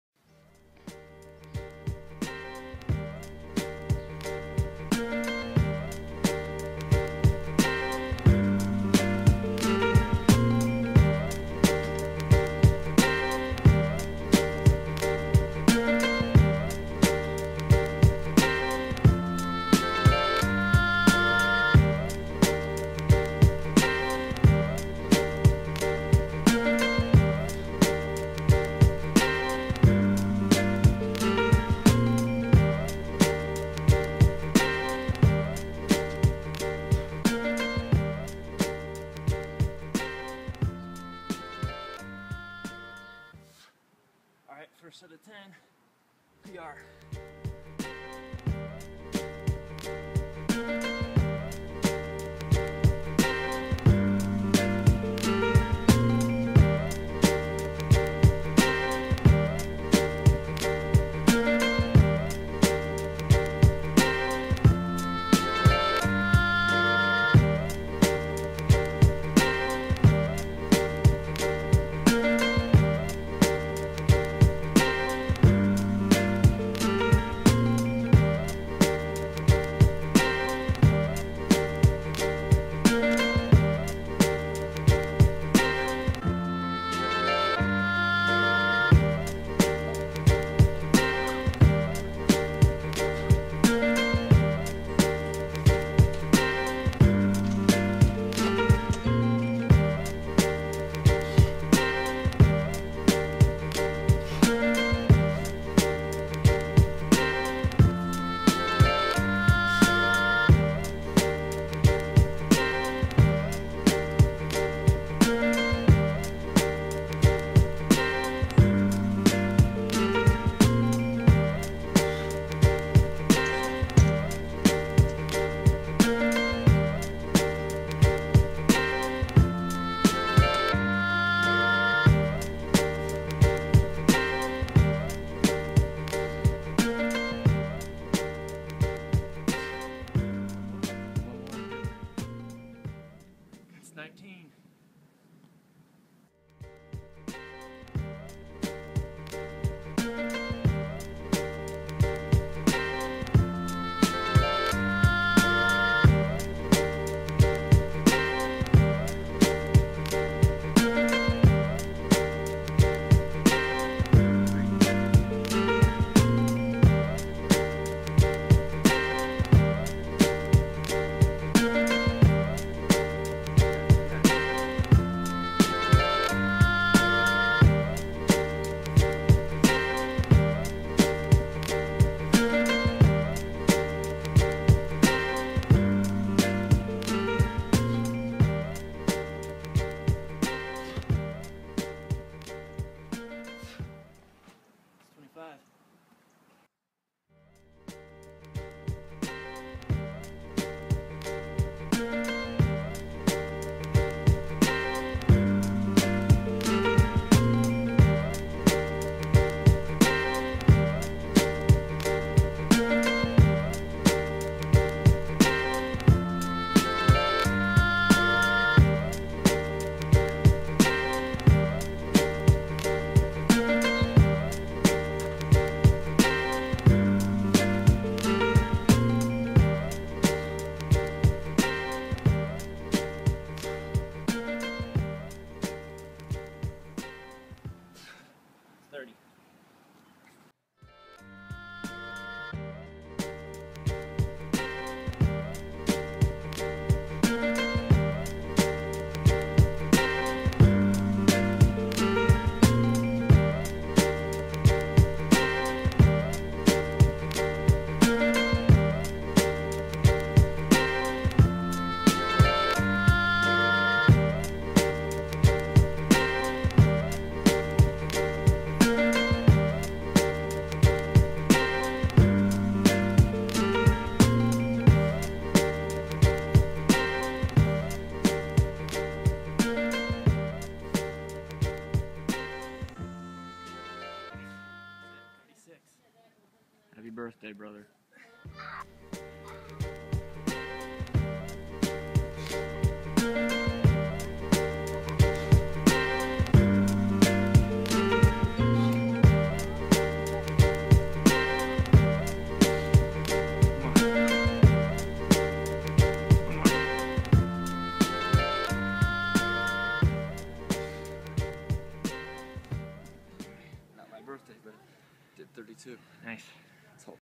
SF BAR 36 Birthday Muscle Ups
36 NON-consecutive muscle ups.Turned 36 a few months back, so I had no choice but to knock-out some birthday m-ups! My brother and I alternated sets with as little rest in between as possible. Tried to throw in a few variations.

muscle-ups, pull-ups, bar workout, calisthenics